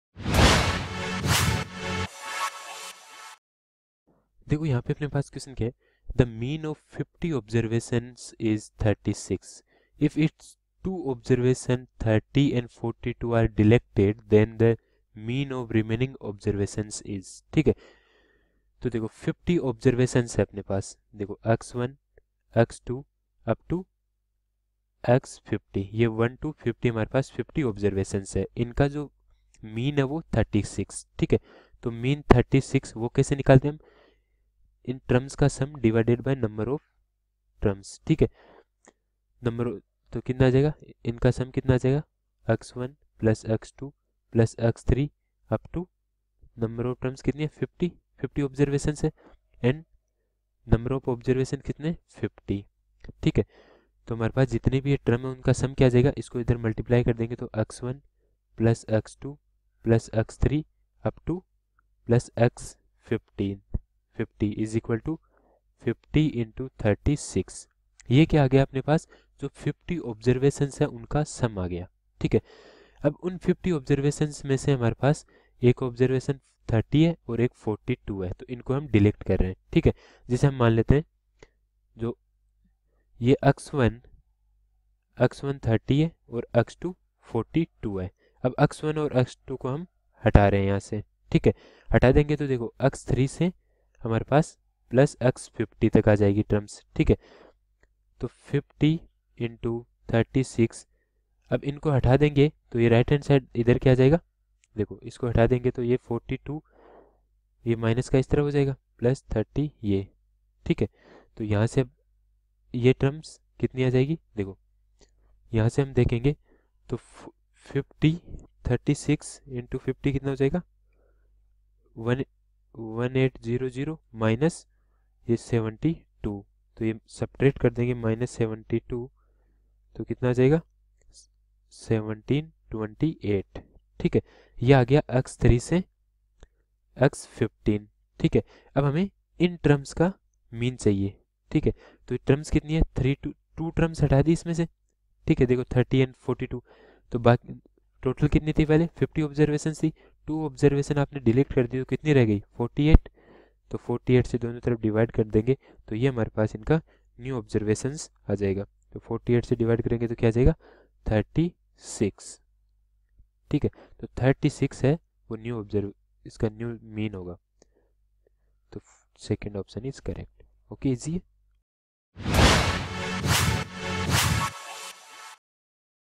The mean of `50` observations is `36`. If its two observations `30` and `42` are deleted, the
To ask Unlimited Maths doubts download Doubtnut The mean of `50` observations is `36`. If its two observations `30` and `42` are deleted, then the mean of the remaining observations is